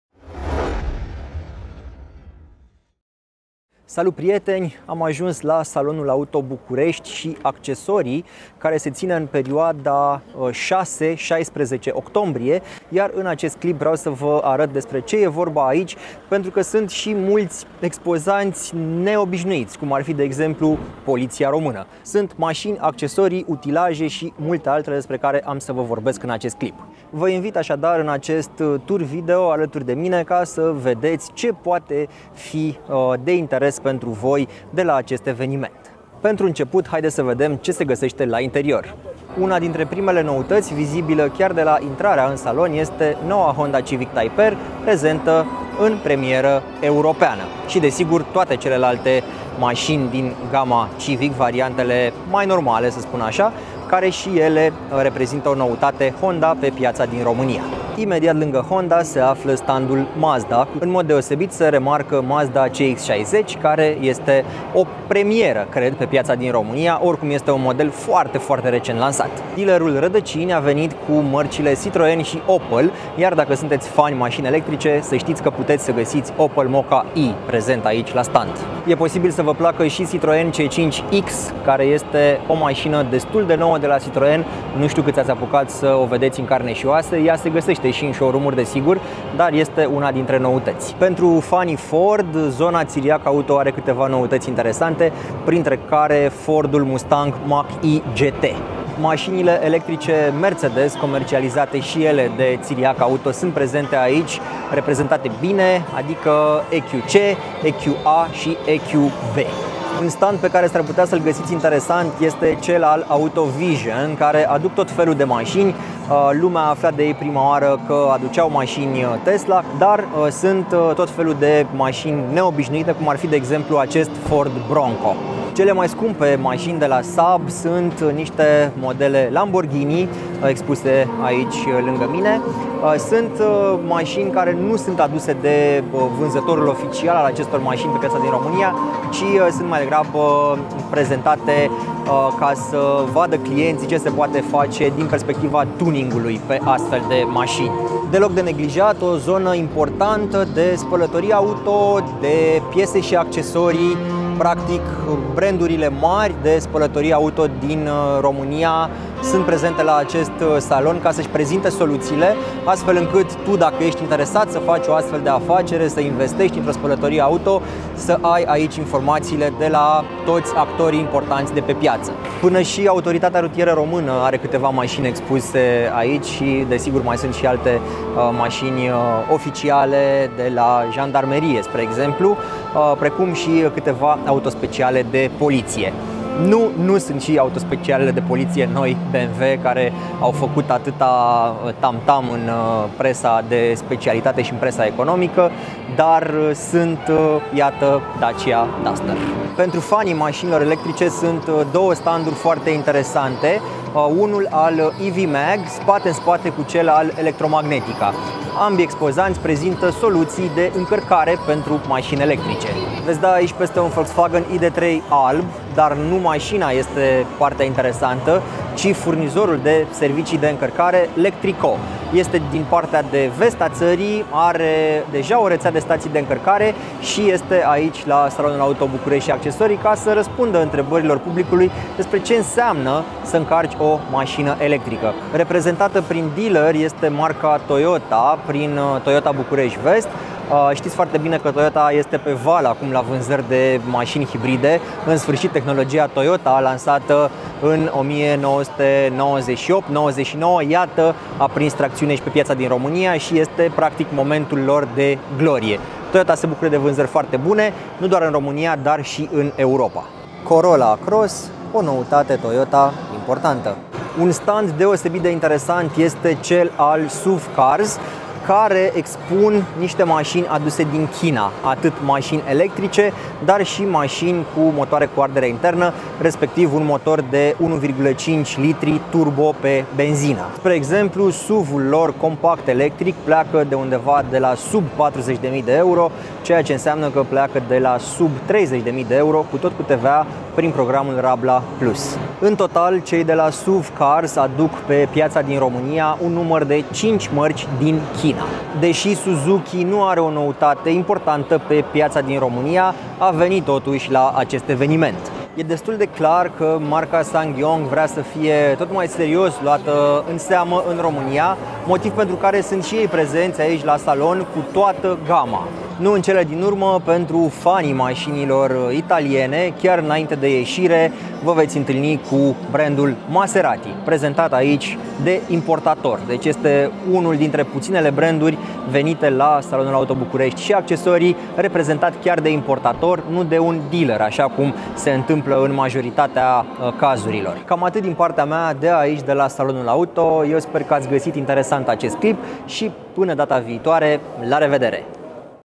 Salonul Auto București & Accesorii 2022 - tur video - Adrian Mitrea / 0-100.ro
Salonul Auto București & Accesorii 2022 se ține în perioada 6-16 Octombrie, la Romexpo. Iată un tur video de la eveniment cu jurnalistul auto Adrian Mitrea.

O galerie foto de la eveniment și informații conexe sunt disponibile aici